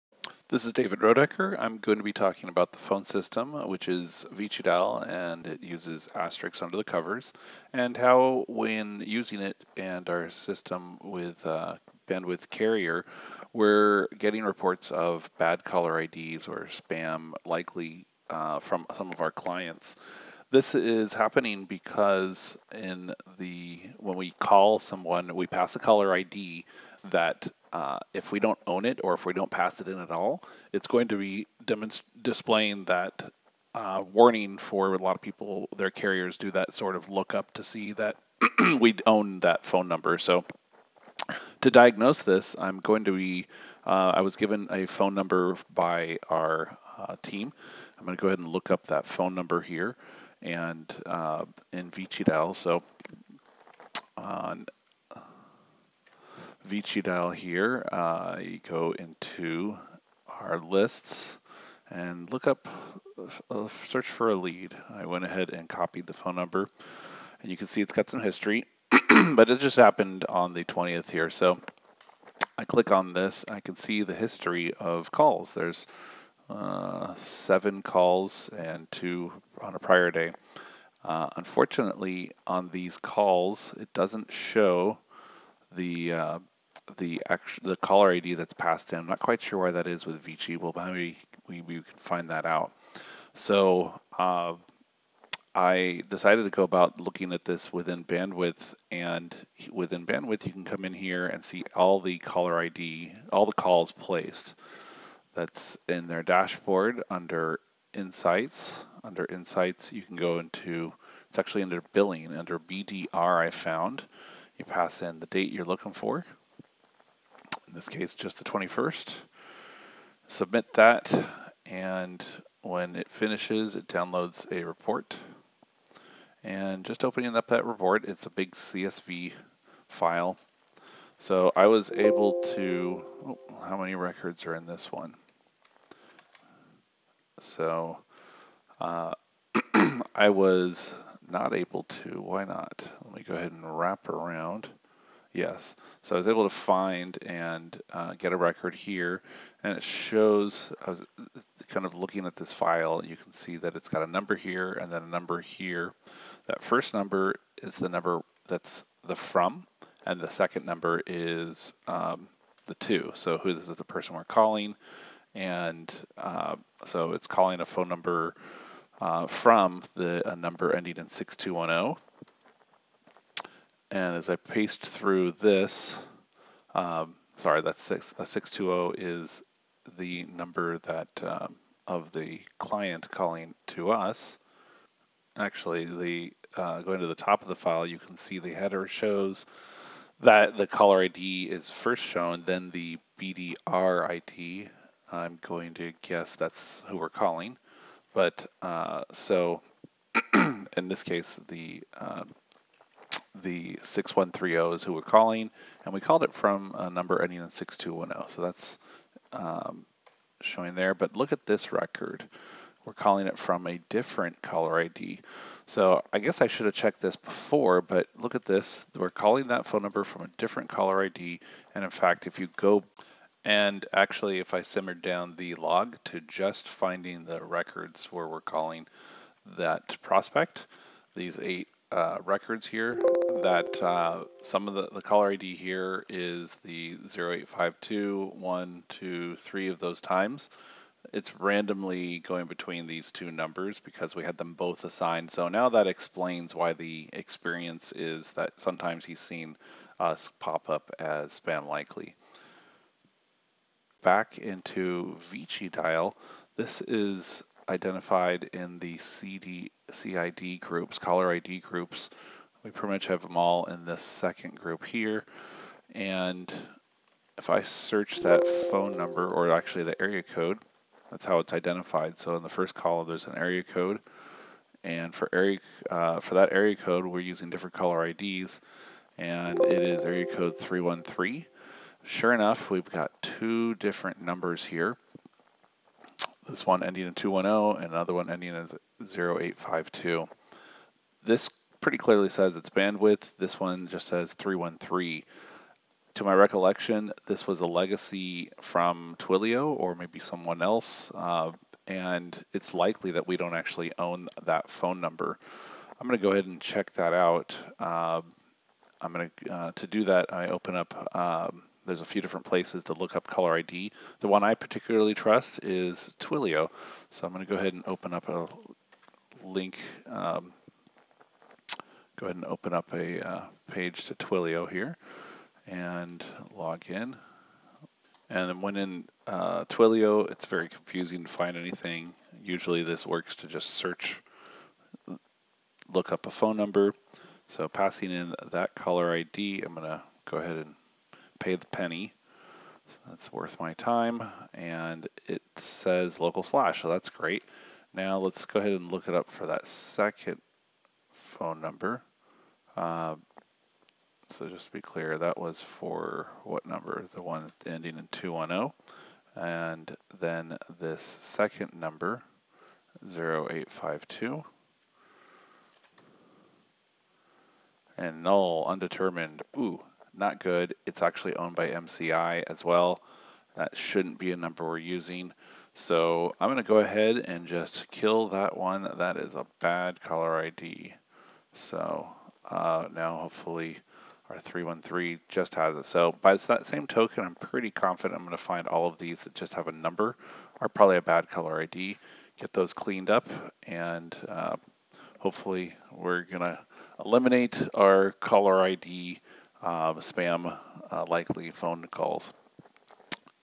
Vicidial Caller ID
When our outbound calls show up as Spam likely, it might be due to our system not signalling out the correct caller ID.
Call data records showing the callerID used are shown in the Bandwidth dashboard as BDR (billing data record).
Further, the value of the caller ID can be looked up Twilio Lookup console.
In Vicidial, the CID is set by area code.